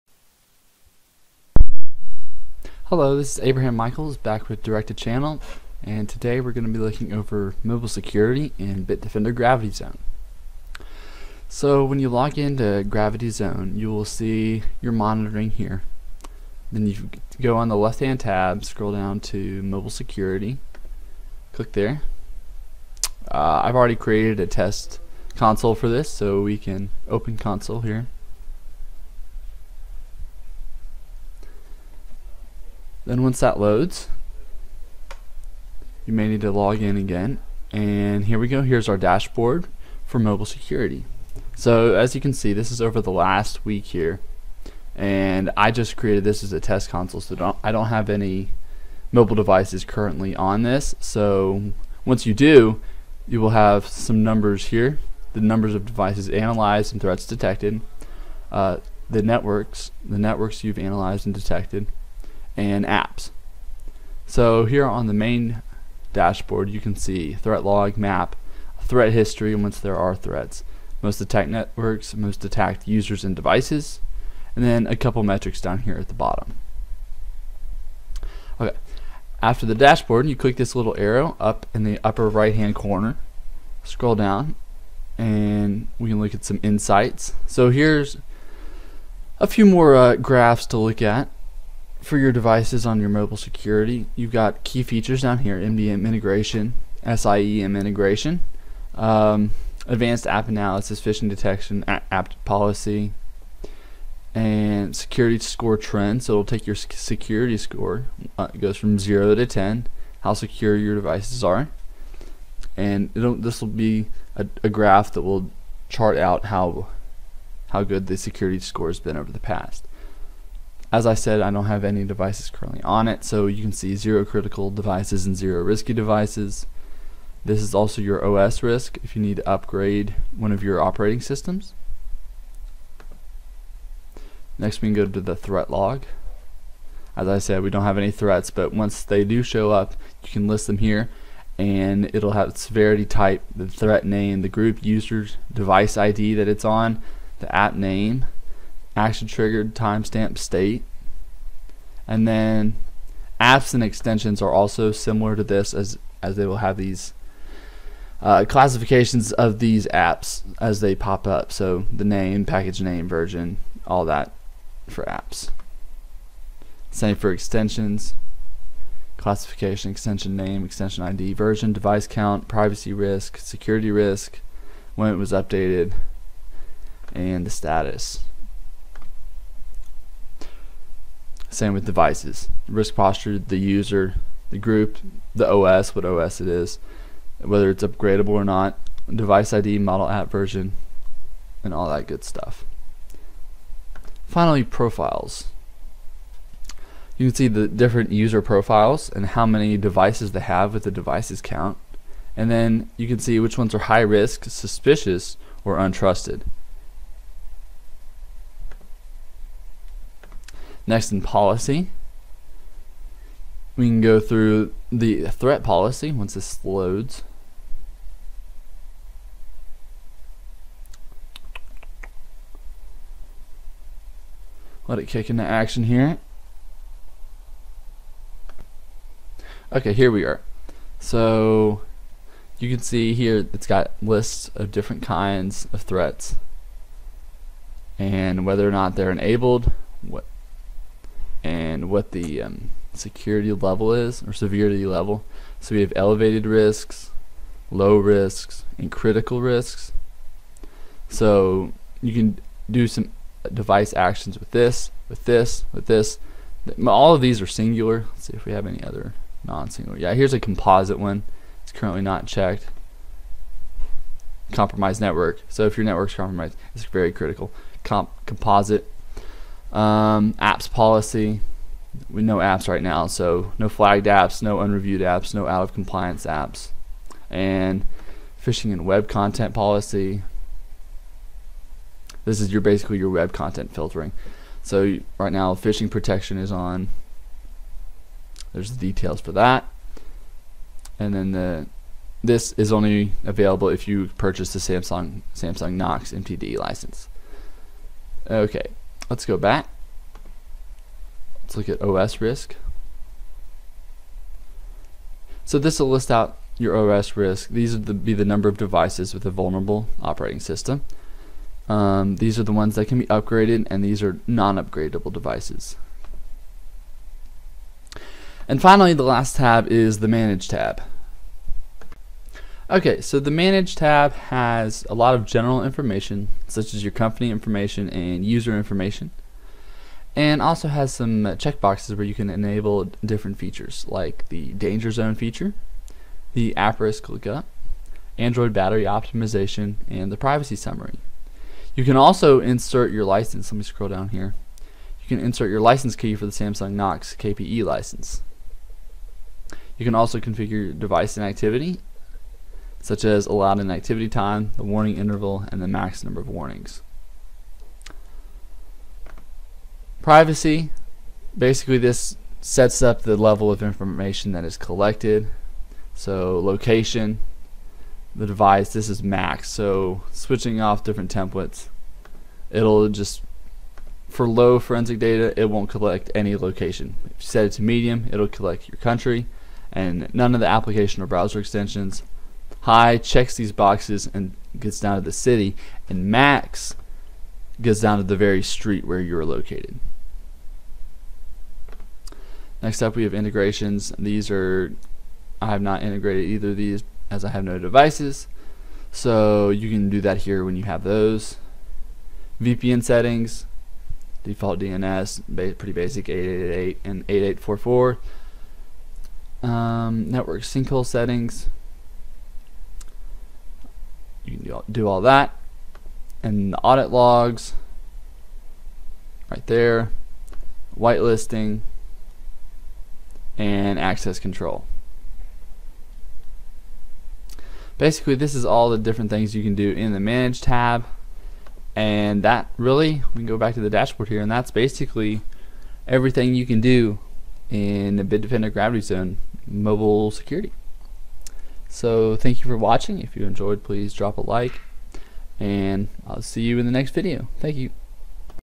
Brief Overview of Mobile Security
Abraham from Direct 2 Channel gives a brief walkthrough of the abilities and features of Bitdefender GravityZone's Mobile Security.